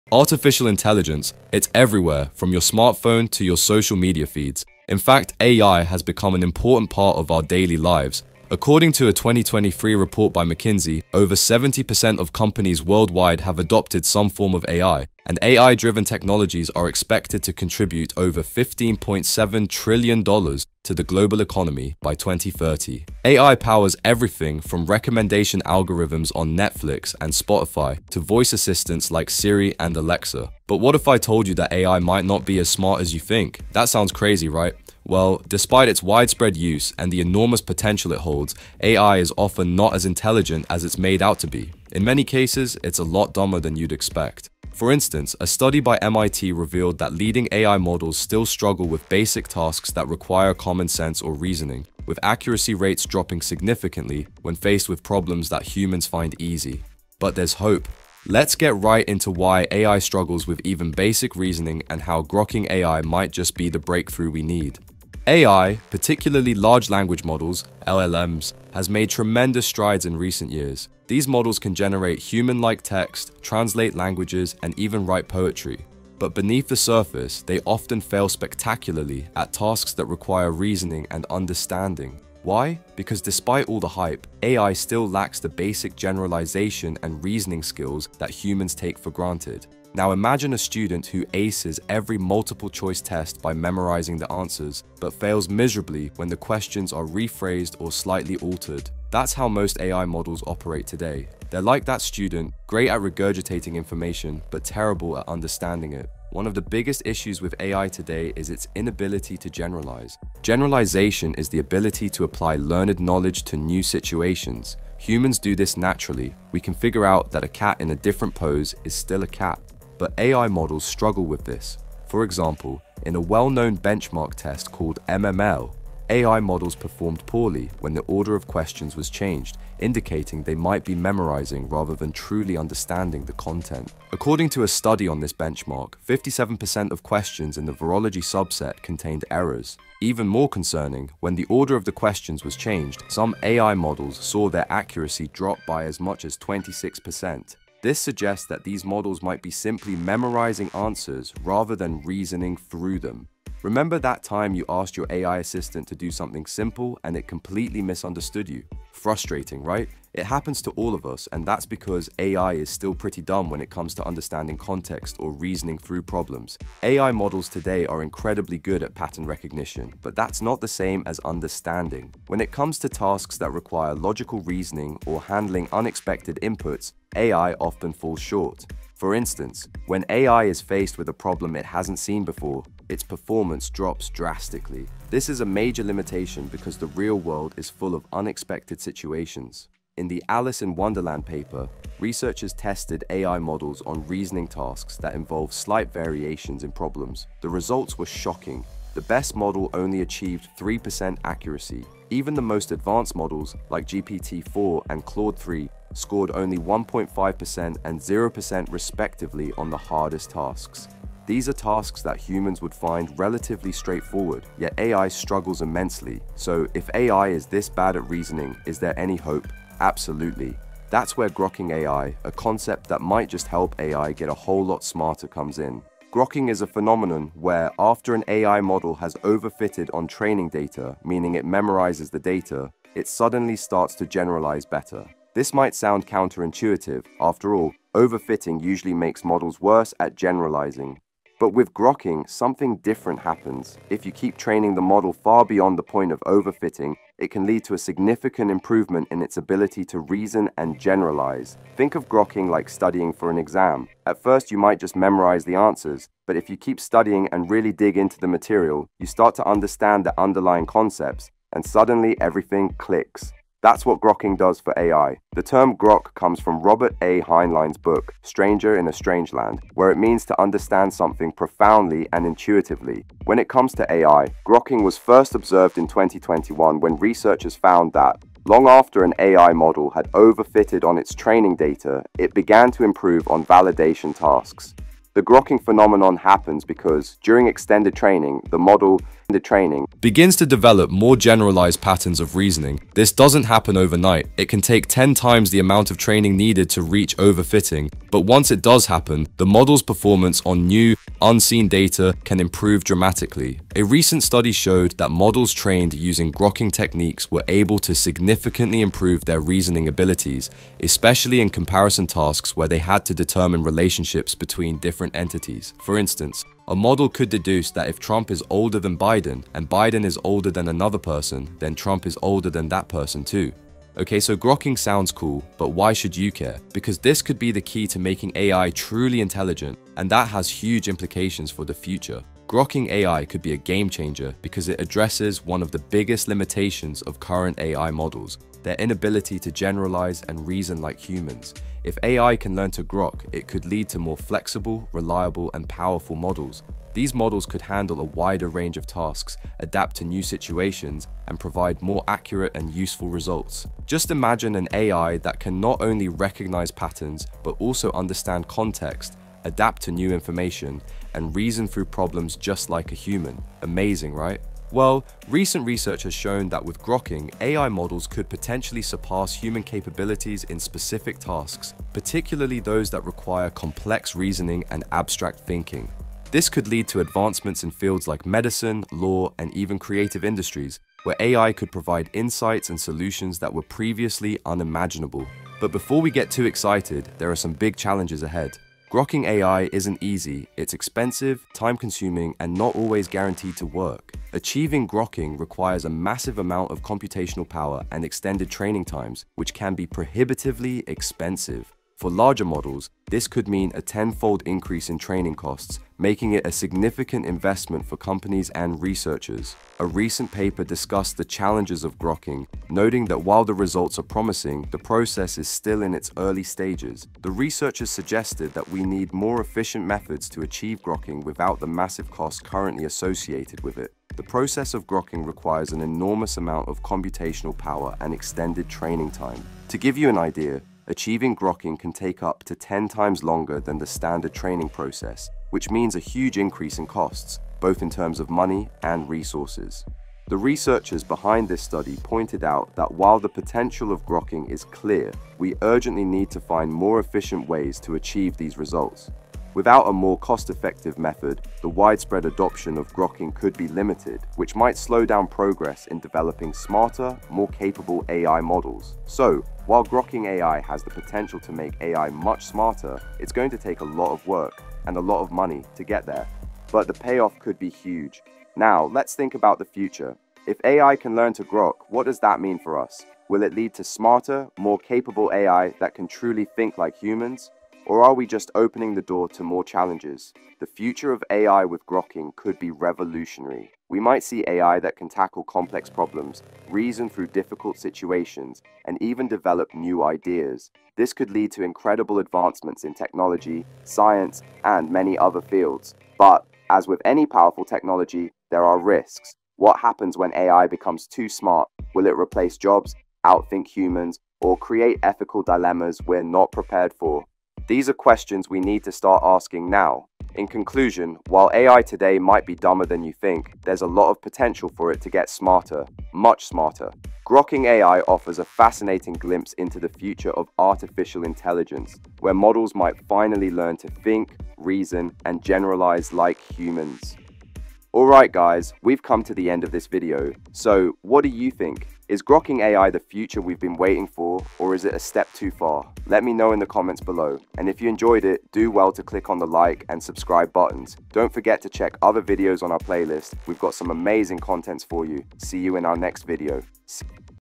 Why AI Is Dumber Than You Think… (And How It Might Get Smarter with Grokking AI)
Despite its widespread use and the enormous potential it holds, AI is often not as intelligent as it’s made out to be. In many cases, it’s a lot dumber than you’d expect.

🔍  Welcome to TECH, your one-stop destination for staying on top of the latest advancements in artificial intelligence, emerging technology, and developing digital skills.